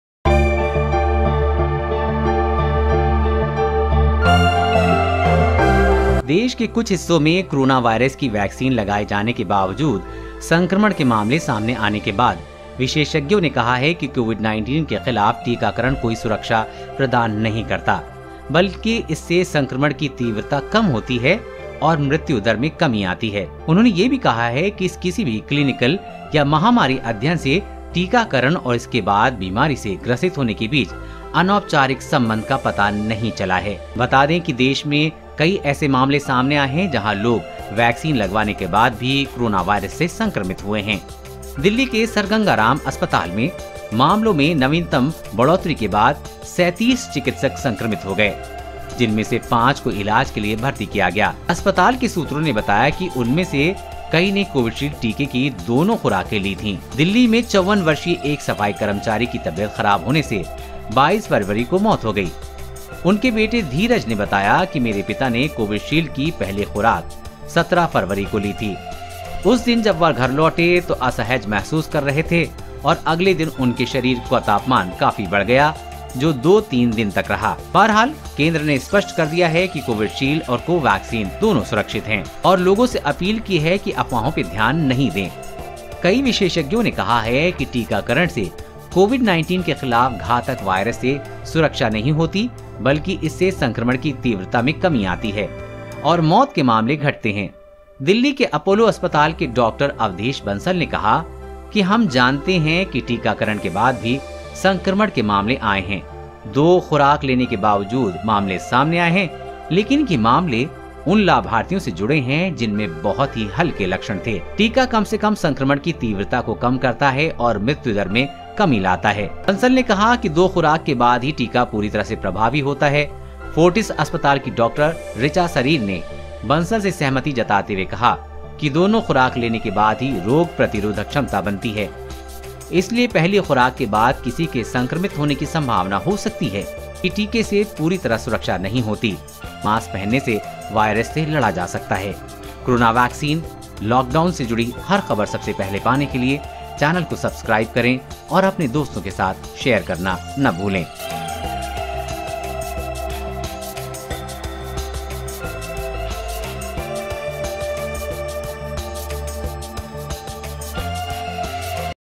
एक्सपर्ट ने बताया क्यों, वैक्सीन कोरोना से नहीं बचा सकती..!!
Corona Vaccine : Vaccine Cannot Save From Corona, Expert Explains The Reason | Corona Virus Case In India | Corona Vaccine Latest Update